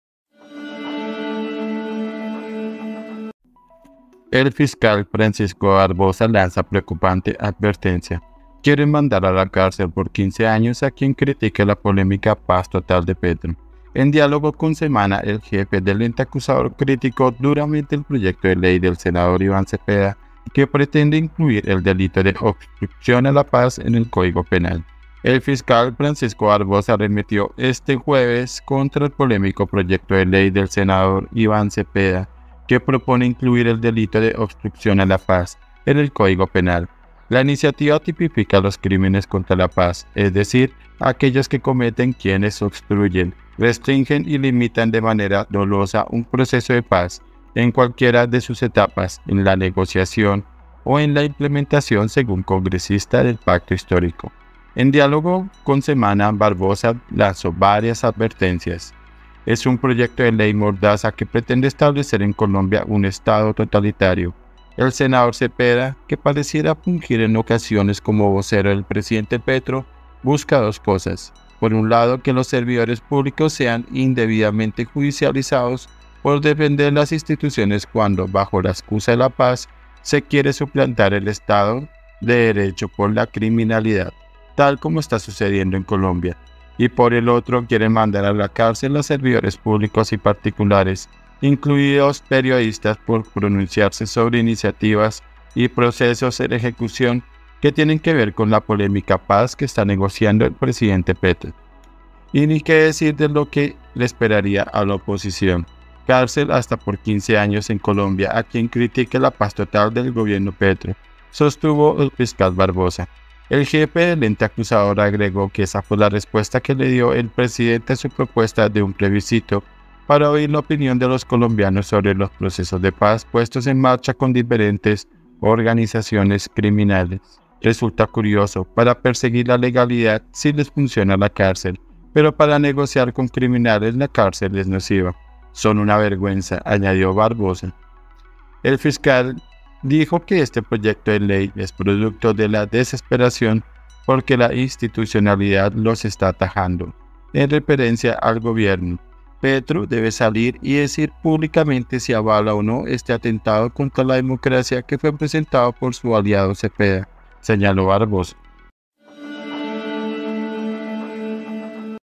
URGENTE ⚠️ EL FISCAL FRANCISCO BARBOSA LANZA PREOCUPANTE ADVERTENCIA
En diálogo con SEMANA, el jefe del ente acusador criticó duramente el proyecto de ley del senador Iván Cepeda que pretender incluir el delito de “obstrucción a la paz” en el Código Penal.
SEMANA